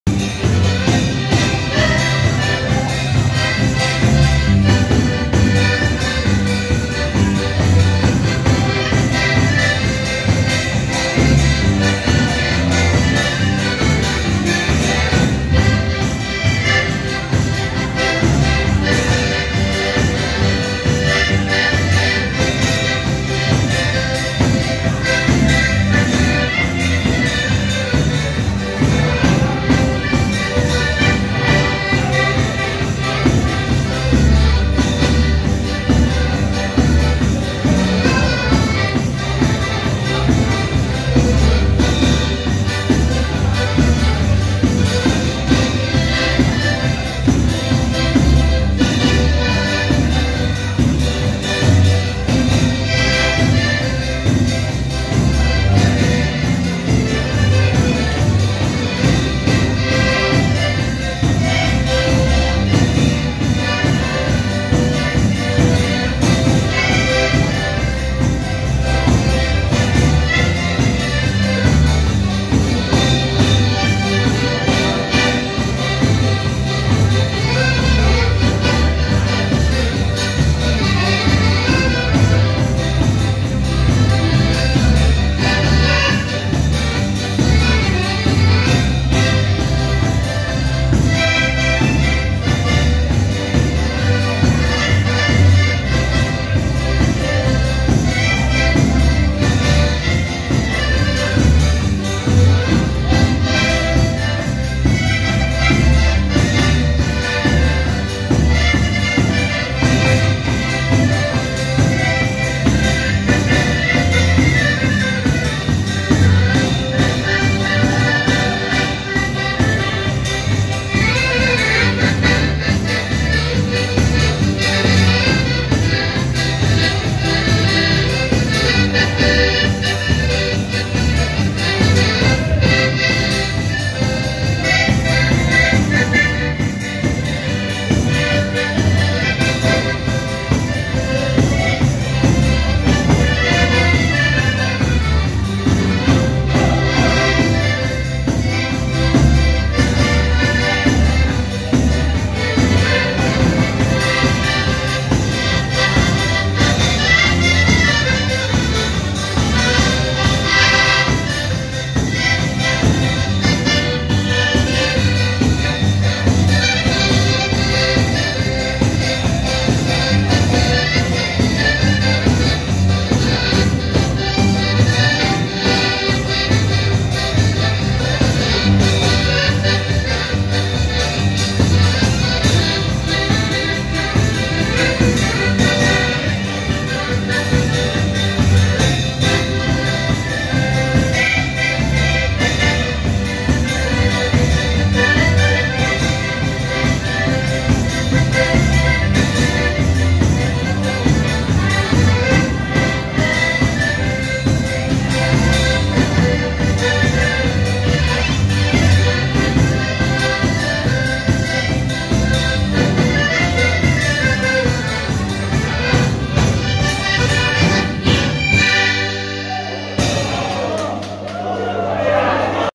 Ludowo i wesoło:Poleczka zespołu "Roland "na rozkręcenie zabawy ostatkowej w Wygnanowie.
Dziękujemy organizatorom (Małgosi,Zosi i sołtysowi Czesiowi)za zorganizowanie udanej imprezy.